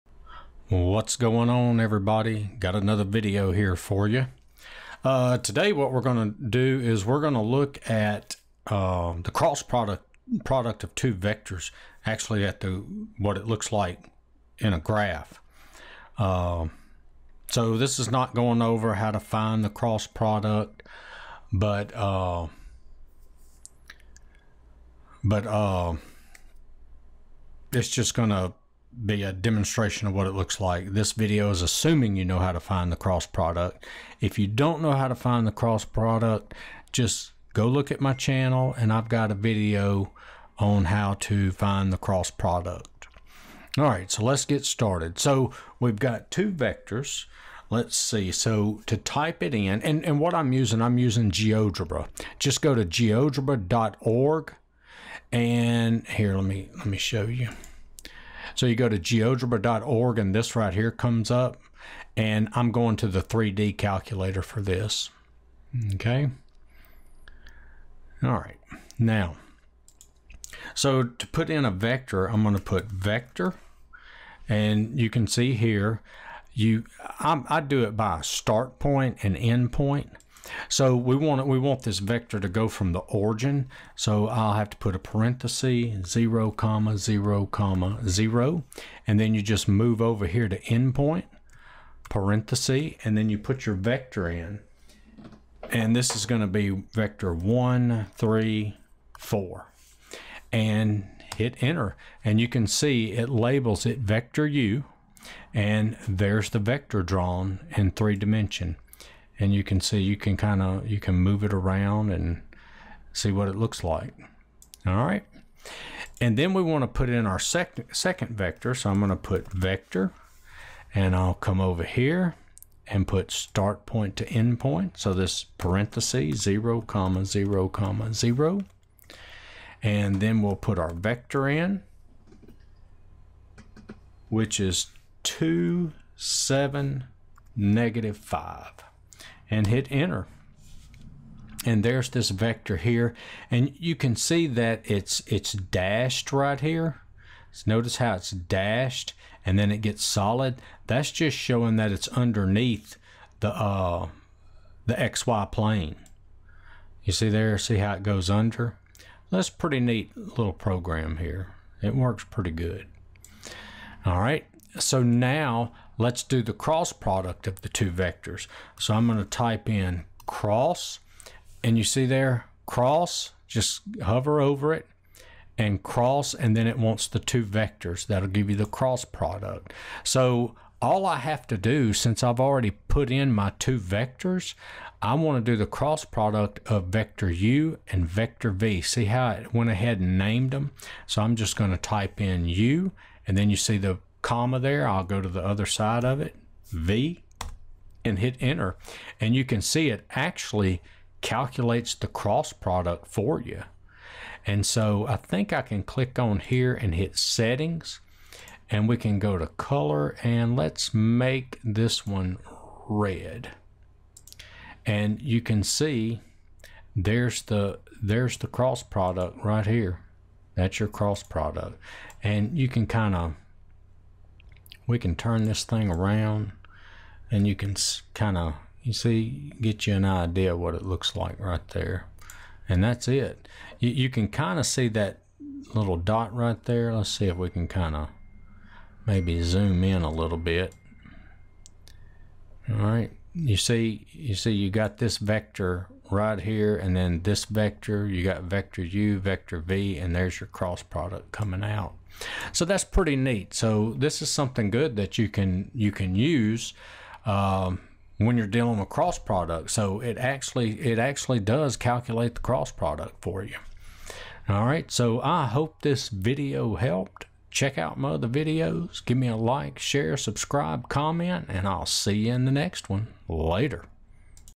Vector Cross Product Visualized In Geogebra
This video shows how to find the cross product and graph it in geogebra.